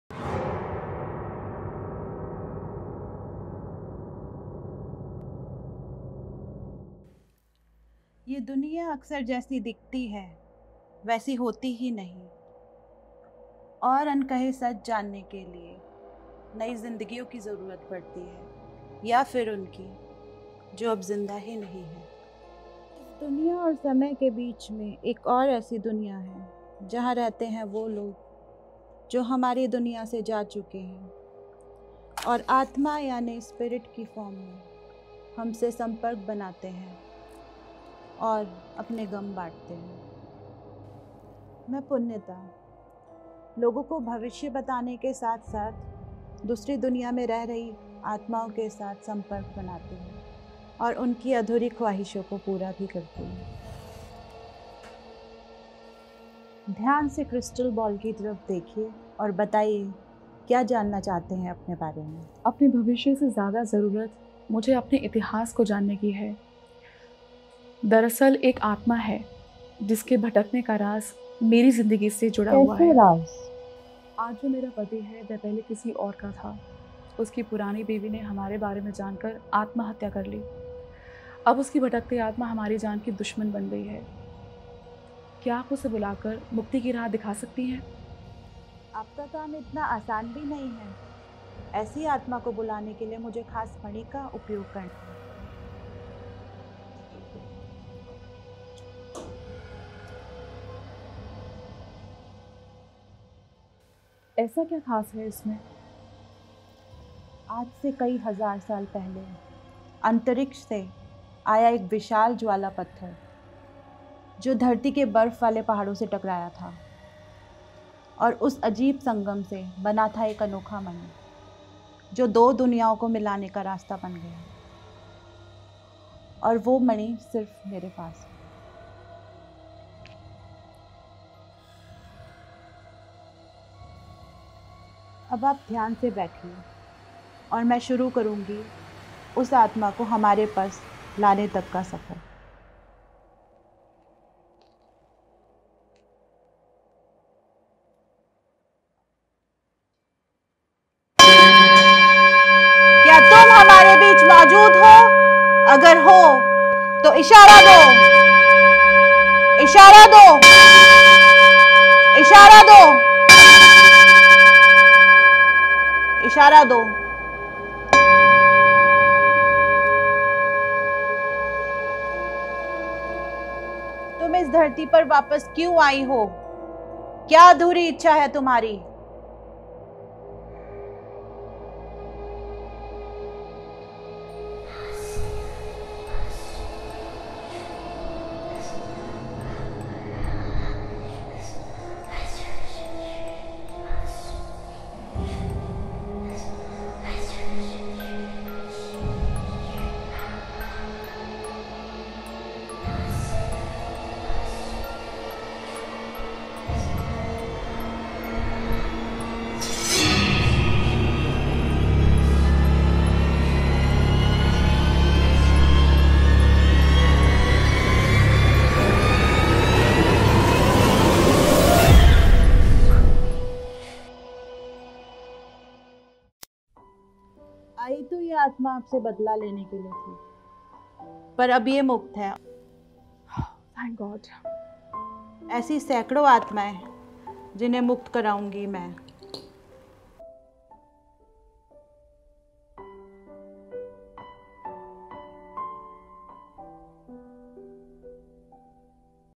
The Unrealistic Real Story | Awsme Productions | Nitiz Joshi | Punyata jain - Tarot Card Reader
The unrealistic real story of a women who is in suffering and wants to have a solution . So she visited Punyata Jain , A Tarot Card reader and Soul handling expert .
and cont...

Production House - Awsme Productions Pvt. Ltd.

Design Your Future by Punyata Jain

Tarot Reader & Spiritual Healer

Cast- Punyata Jain and Shalini Marwah
Director - Shubham Dwivedi
Production Head / D.O.P - Nitiz Joshi
Camera Man - Yatin
Assistant Director - Sachin
Makeup by - Piyuksha